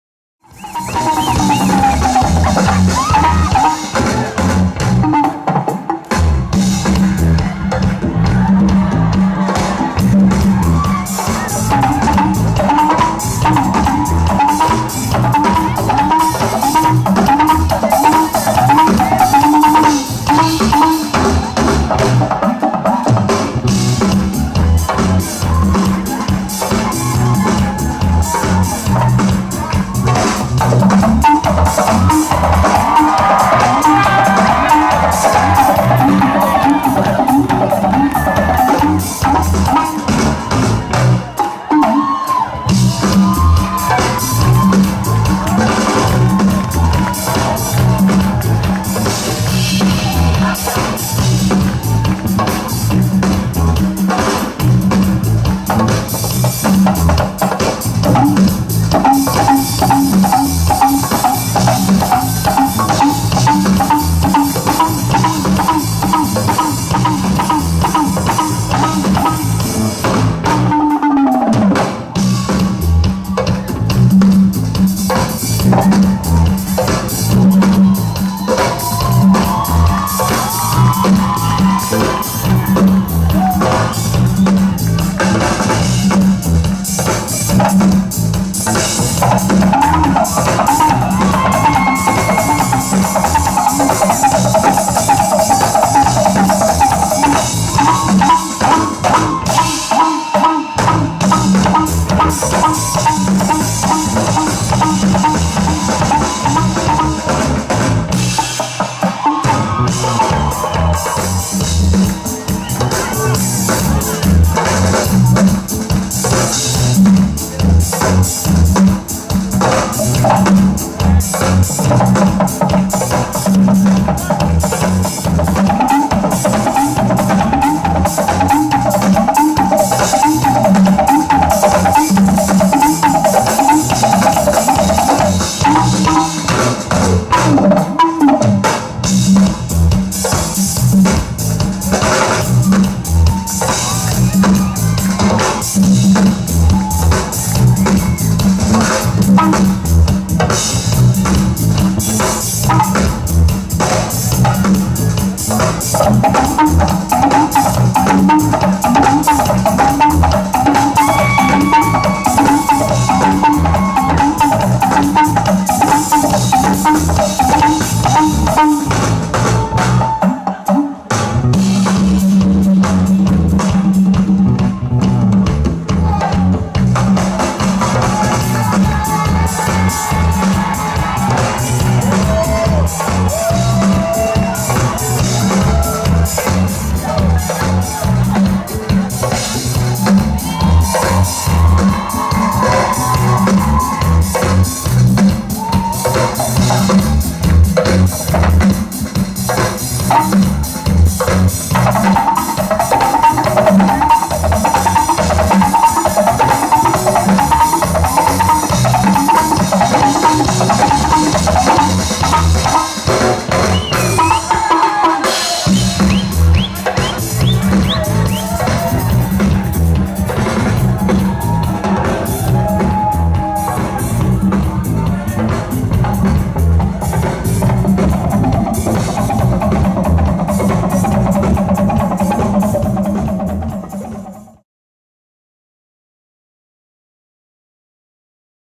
Dance-Off with ANGELIQUE KIDJO: Jazz and Creole Festival 2014
ANGELIQUE KIDJO graced the Jazz and Creole Festival 2014 stage at the Cabrits National Park in Dominica. Reelpositivevibes brings it to you from BrBpTV.